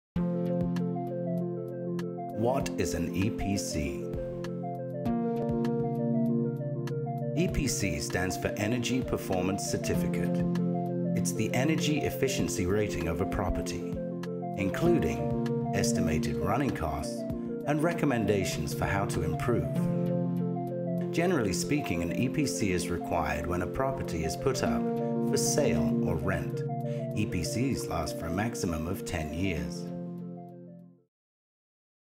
What Is an EPC?
EPC stands for Energy Performance Certificate. New guidance has been published recently by the UK government to assist landlords in understanding the new regulations for energy efficiency.